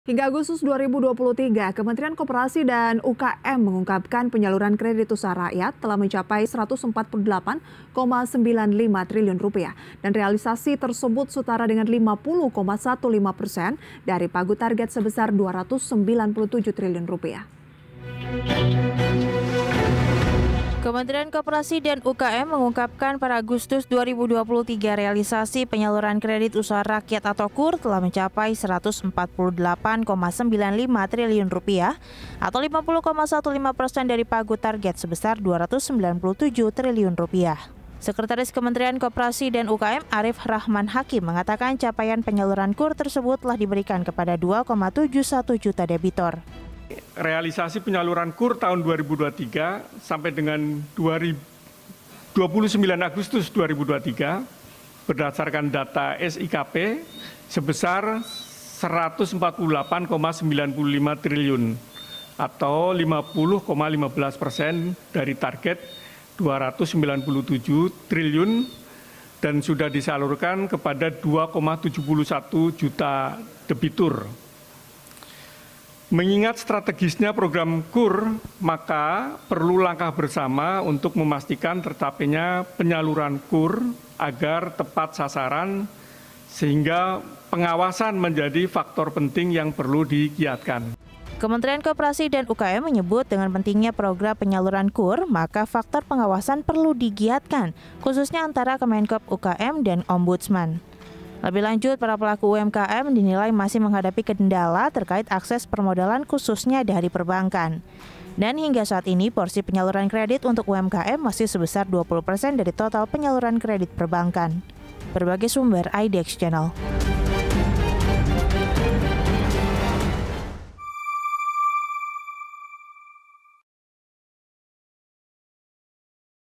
Per Agustus 2023, Penyaluran KUR Capai Rp148,95 T | IDX CHANNEL
Hingga Agustus 2023, Kementerian Koperasi dan UKM mengungkapkan penyaluran kredit usaha rakyat (KUR) telah mencapai Rp148,95 triliun. Realisasi tersebut setara dengan 50,15% dari Pagu target sebesar Rp297 triliun.